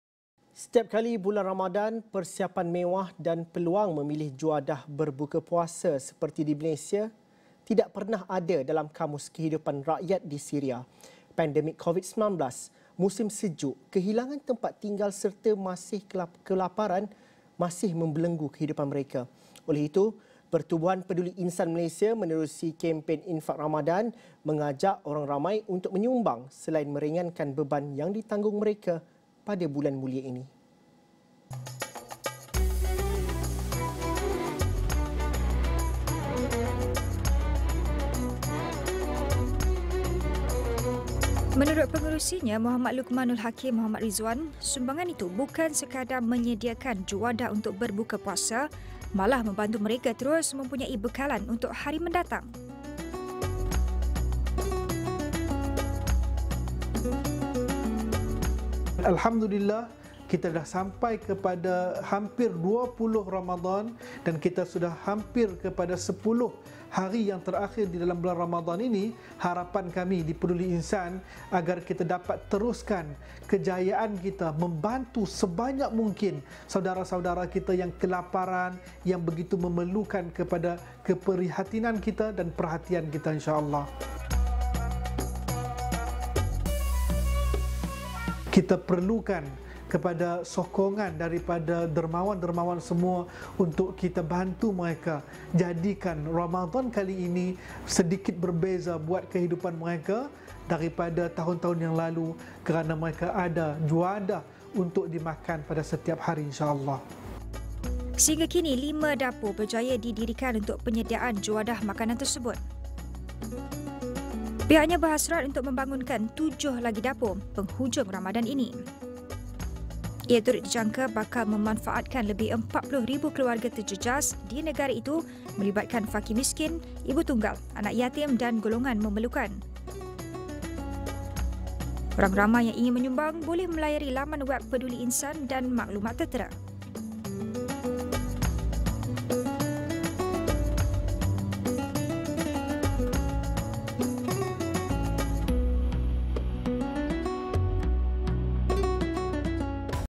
Peduli Insan | Bantu pelarian Syria sambut Ramadan
Pandemik COVID-19, musim sejuk, kehilangan tempat tinggal serta kelaparan masih membelenggu rakyat Syria. Oleh itu, Pertubuhan Peduli Insan Malaysia menerusi 'Kempen Infaq Ramadan Syria' mengajak orang ramai untuk menyumbang selain meringkan beban ditanggung mereka pada bulan mulia ini.